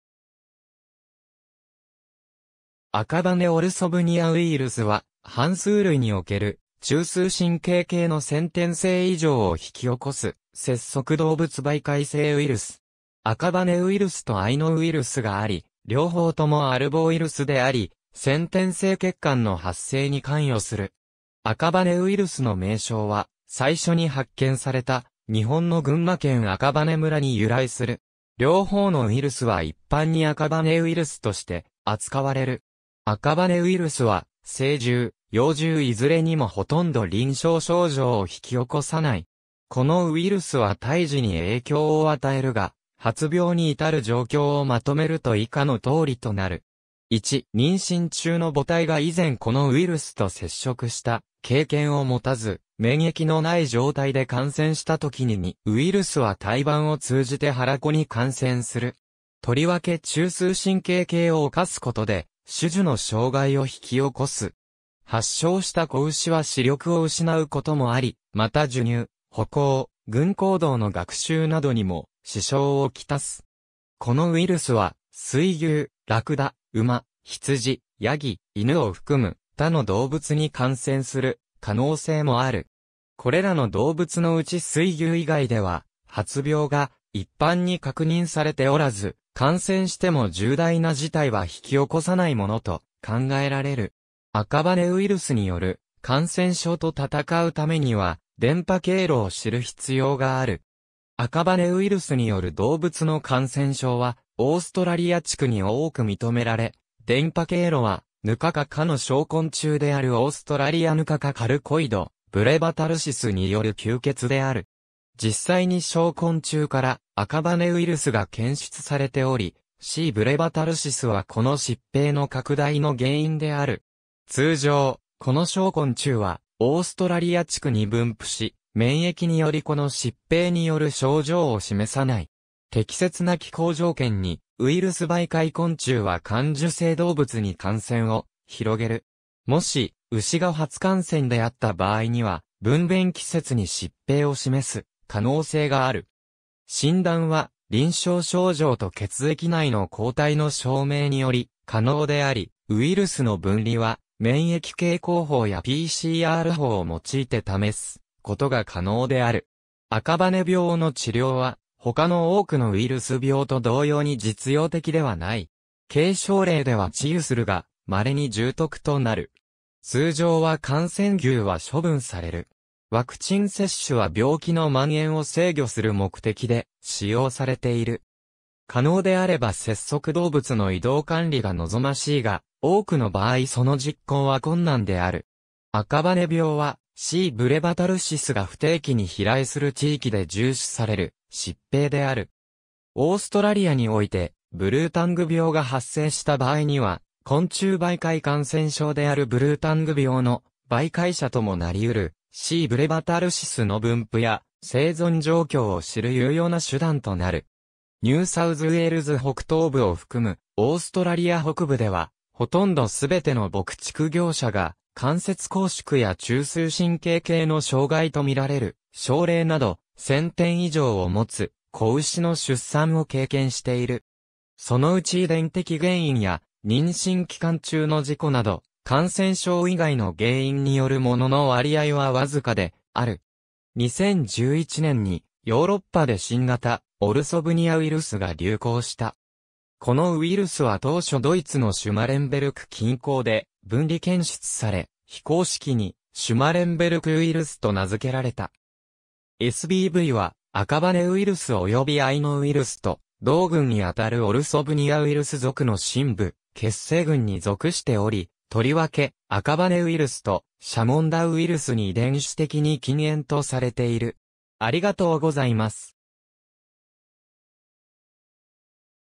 アカバネウイルス
アカバネ・オルソブニヤウイルス (英: Akabane orthobunyavirus)は、反芻類における中枢神経系の先天性異常を引き起こす節足動物媒介性ウイルス。
アカバネウイルスとアイノウイルスがあり、両方ともアルボウイルスであり、先天性欠陥の発生に関与する。
アカバネウイルスの名称は最初に発見された日本の群馬県赤羽村（現:館林市）に由来する。
両方のウイルスは一般にアカバネウイルスとして扱われる。
アカバネウイルスは成獣、幼獣いずれにもほとんど臨床症状を引き起こさない。
このウイルスは胎児に影響を与えるが、発病に至る状況をまとめると以下の通りとなる。
1.妊娠中の母胎が以前このウイルスと接触した経験を持たず、免疫のない状態で感染したときに　2.ウイルスは胎盤を通じて胎子に感染する。
とりわけ中枢神経系を侵すことで種々の障害を引き起こす。
発症した子牛は視力を失うこともあり、また授乳、歩行、群行動の学習などにも支障をきたす。
このウイルスはスイギュウ、ラクダ、ウマ、ヒツジ、ヤギ、イヌを含む他の動物に感染する可能性もある。
これらの動物のうちスイギュウ以外では発病が一般に確認されておらず、感染しても重大な事態は引き起こさないものと考えられる。
アカバネウイルスによる感染症と戦うためには伝播経路を知る必要がある。
アカバネウイルスによる動物の感染症はオーストラリア地区に多く認められ、伝播経路はヌカカ科の小昆虫であるオーストラリアヌカカ"Culicoides brevitarsis"による吸血である。
実際に小昆虫からアカバネウイルスが検出されており、C. brevitarsisはこの疾病の拡大の原因である。
通常、この小昆虫はオーストラリア地区に分布し、免疫によりこの疾病による症状を示さない。
適切な気候条件（温かい湿気の多い夏）にウイルス媒介昆虫は感受性動物に感染を広げる。
もし、ウシが初感染であった場合には分娩季節に疾病を示す可能性がある。
診断は臨床症状と血液内の抗体の証明により可能であり、ウイルスの分離は免疫蛍光法やPCR法を用いて試すことが可能である。
アカバネ病の治療は他の多くのウイルス病と同様に実用的ではない。
軽症例では治癒するが、稀に重篤となる。
通常は感染牛は処分される。
ワクチン接種は病気の蔓延を制御する目的で使用されている。
可能であれば節足動物の移動管理が望ましいが、多くの場合その実行は困難である。
アカバネ病は、C. brevitarsisが不定期に飛来する地域で重視される疾病である。
オーストラリアにおいてブルータング病が発生した場合には、昆虫媒介感染症であるブルータング病の媒介者ともなりうるC. brevitarsisの分布や生存状況を知る有用な手段となる。
ニューサウズウェールズ北東部を含むオーストラリア北部では、ほとんど全ての牧畜業者が関節拘縮や中枢神経系の障害とみられる症例など、先天異常を持つ仔牛の出産を経験している。
そのうち遺伝的原因や妊娠期間中の事故など、感染症以外の原因によるものの割合はわずかである。
2011年にヨーロッパで新型オルソブニアウイルスが流行した。
このウイルスは当初ドイツのシュマレンベルク近郊で分離検出され、非公式にシュマレンベルクウイルス(SBV)と名付けられた。
SBVは、アカバネウイルスおよびアイノウイルスと同群にあたるオルソブニヤウイルス属のSimbu血清群に属しており、とりわけアカバネウイルスとシャモンダウイルスに遺伝子的に近縁とされている。